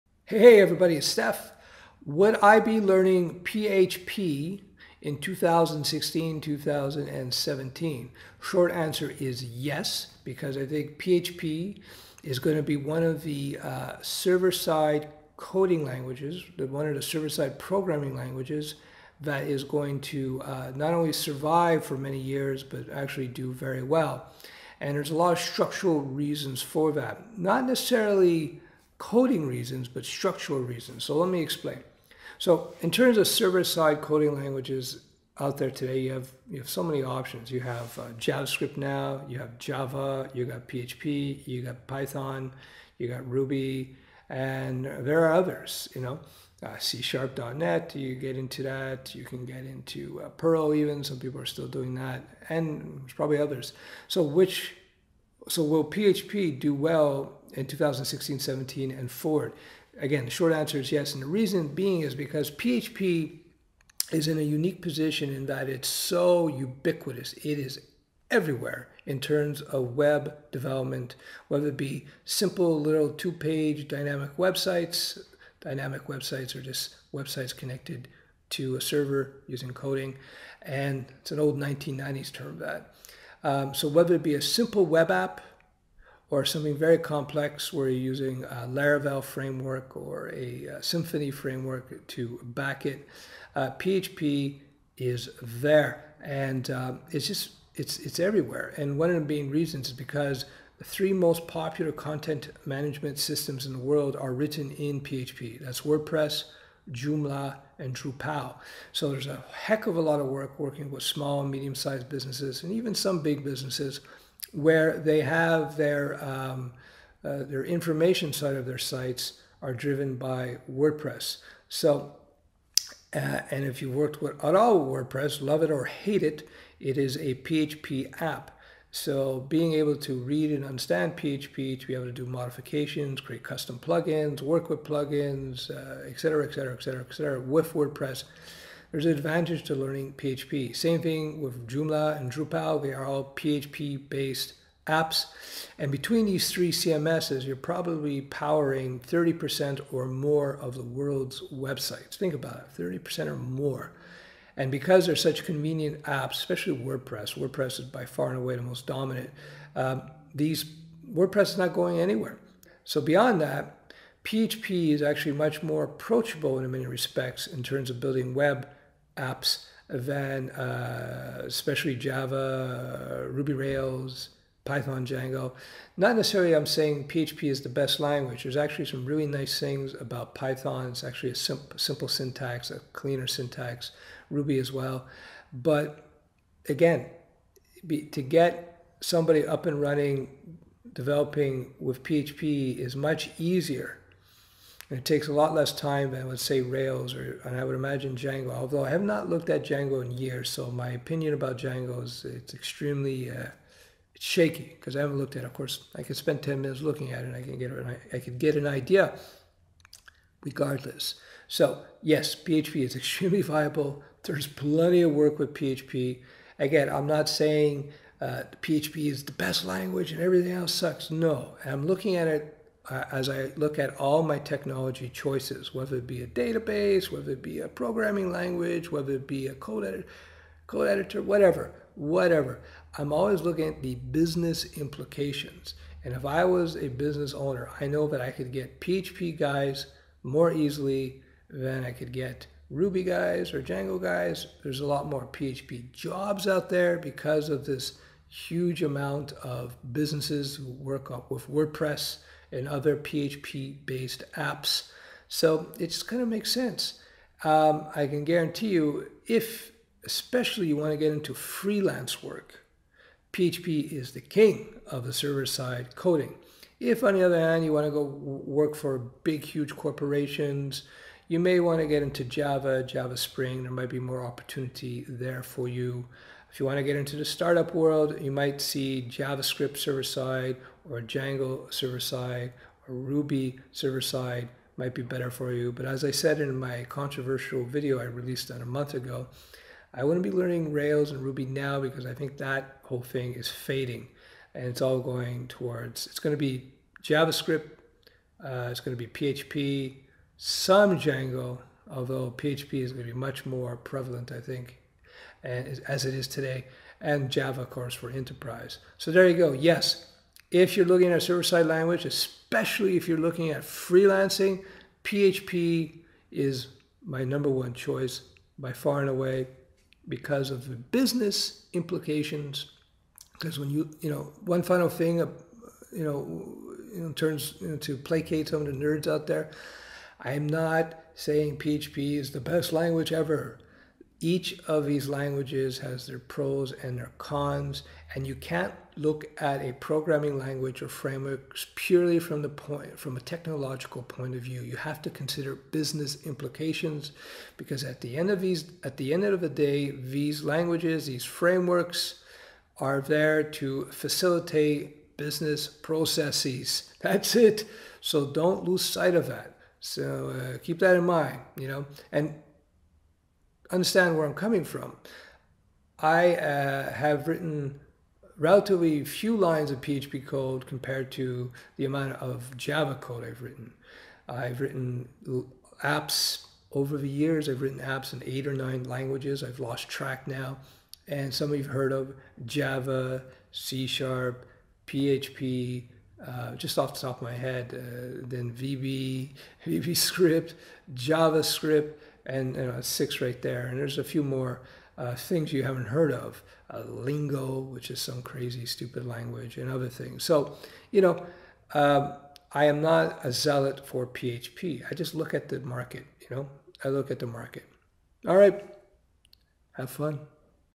Should you learn PHP in 2016 and 2017?
Should you learn PHP in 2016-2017? For freelancers, it's a big yes! Check out my powerful course on PHP 7, JavaScript, SQL and so much more